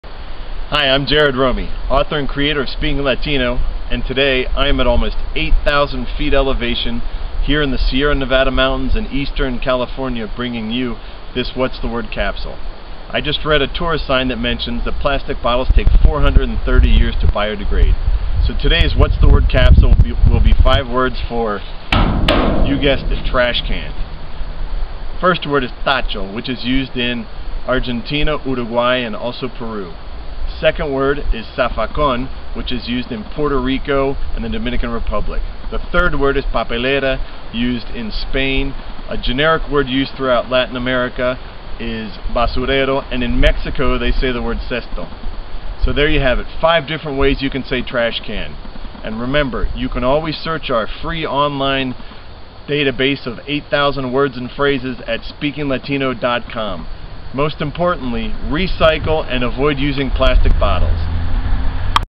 6 Spanish Words for TRASH CAN: How to say TRASH CAN in Spanish
Read the complete blog post "6 Spanish Language Words for TRASH CAN"

Jared Romey from Speaking Latino tells you different Spanish slang words for TRASH CAN and the countries that use them.